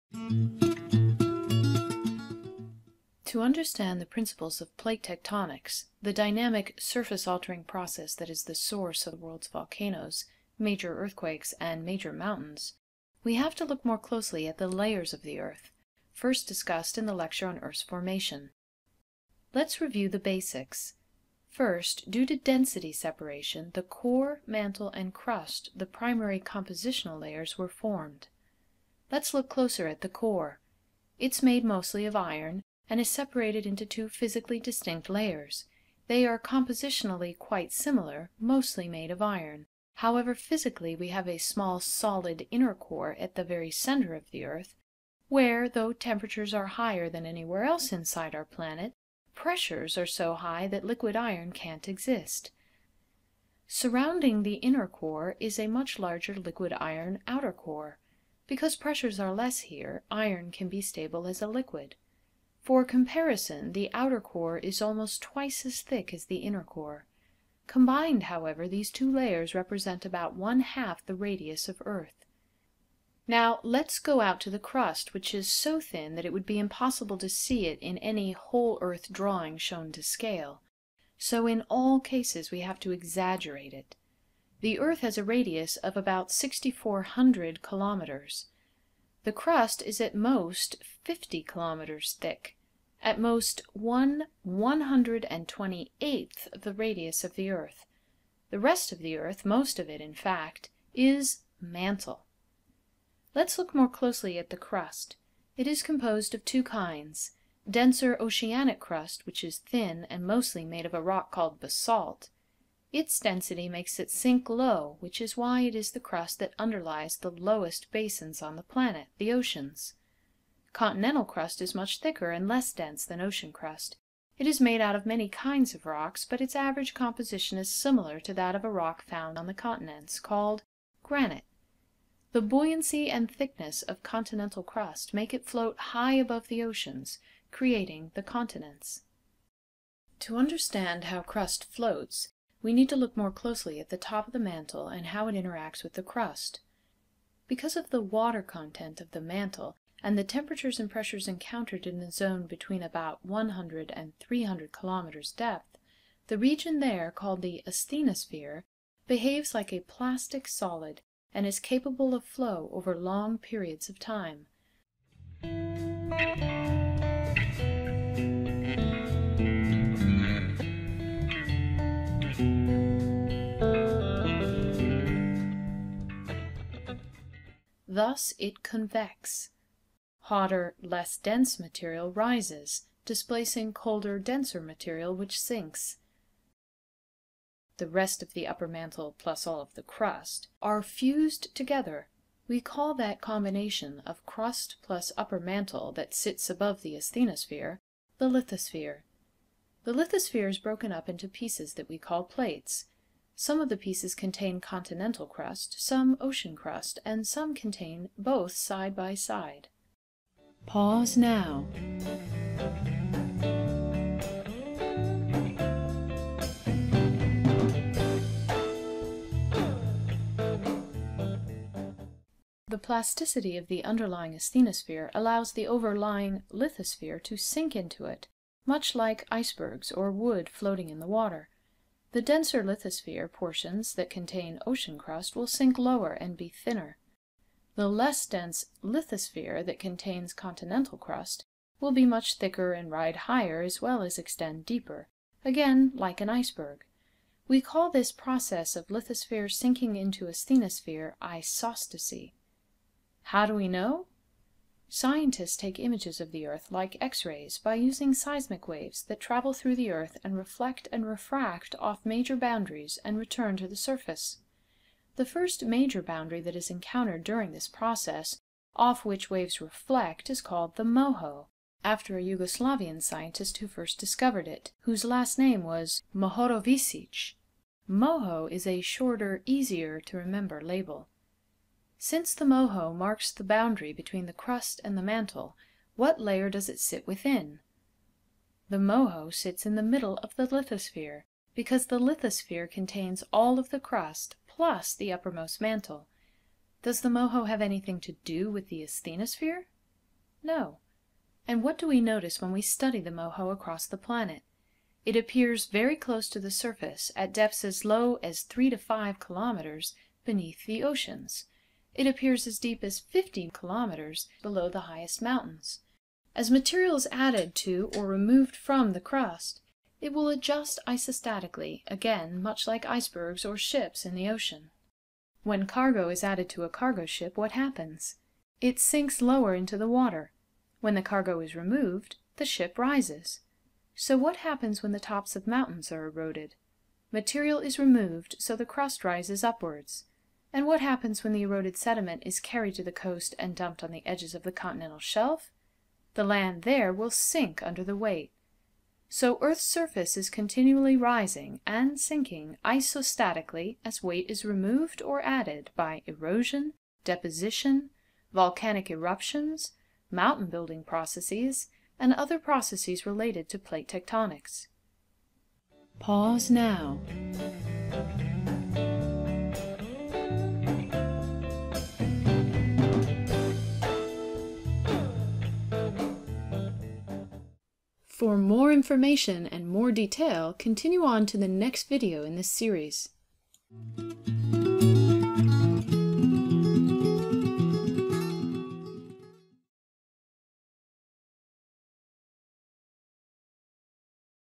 Earth Layers & Isostasy IG8
Earth Layers & Isostasy

For an introductory college-level earth sciences class: a review of Earth's layers -- crust, mantle, inner core, outer core, asthenosphere, lithosphere -- including their thicknesses, compositions, and basic properties (+the description of the moho: the boundary between the crust and mantle). We also discuss how the Earth's crust and lithosphere rise and fall isostatically as weight is added or removed.
** For a full playlist, refer to the Oceanography or Geology playlists on the "aqknowledgehub" YouTube Channel.

Content within this video is based on information available in any standard introductory college oceanography or geology textbook, such as Essentials of Oceanography -- Trujillo and Thurman -- OR Essentials of Geology -- Tarbuck and Lutgens -- Pearson Publishing.